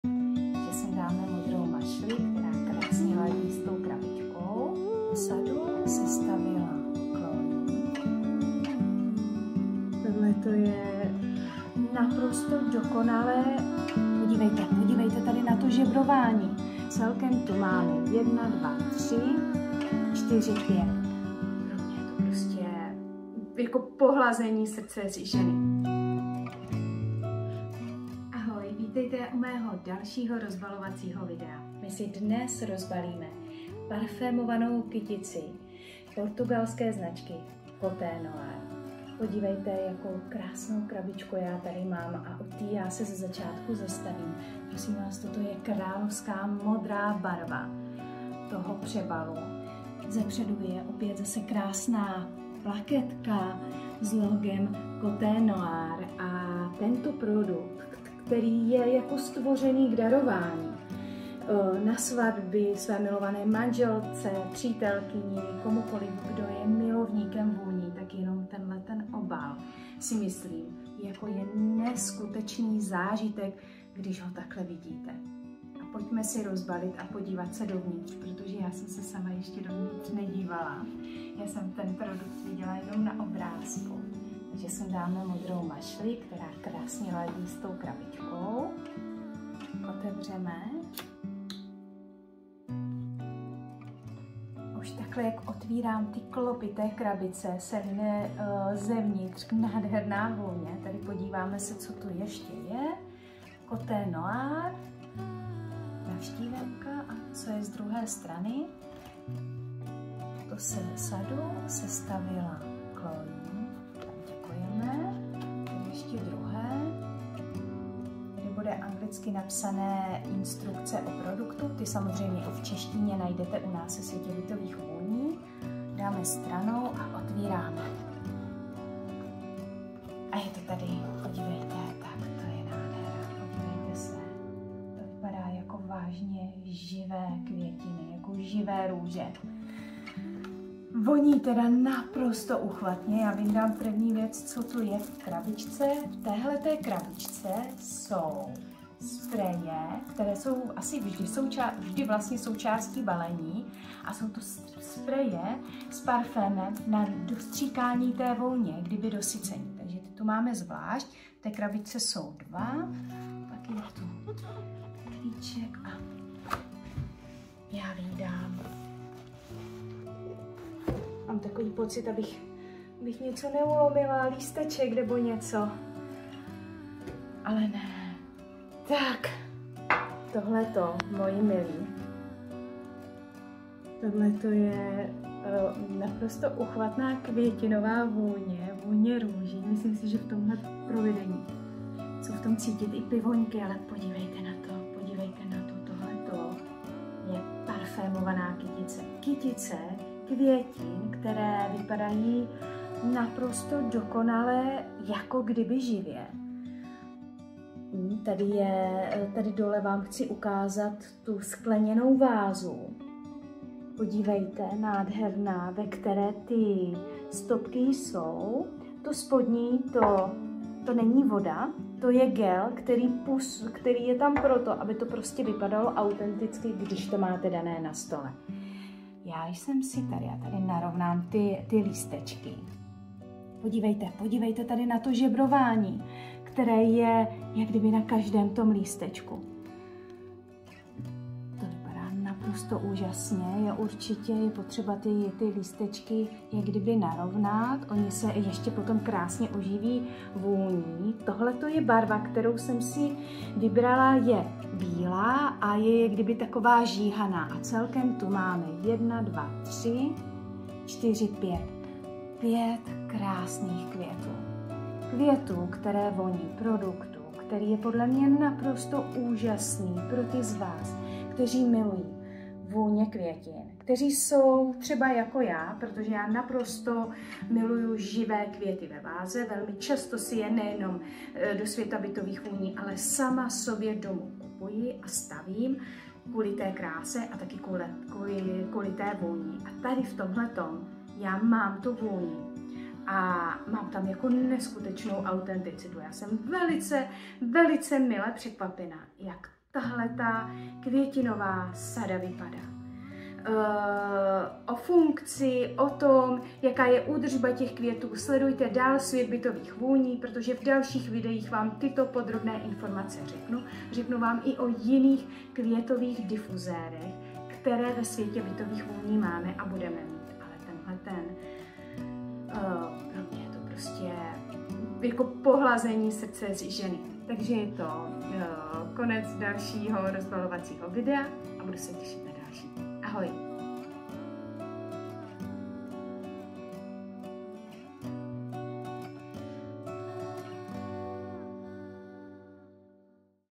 Rozbalení luxusní kytice parfémovaných růží Côte Noire
Luxusní parfémovaná kytice růží elegantně umístěna ve skleněné váze zdobené vzorem Herringbone a prezentovaná v atraktivní boxu se zlatou kovovou plaketou a velkou romantickou mašlí. Každá růže je precizně zpracována a opatřena speciálním povlakem, který dodává okvětním lístkům přirozený vzhled. Součástí balení jsou dva prvotřídní francouzské parfémy Pink Peony a Rose Oud, které se aplikují přímo na růže dle preferované intenzity vůně.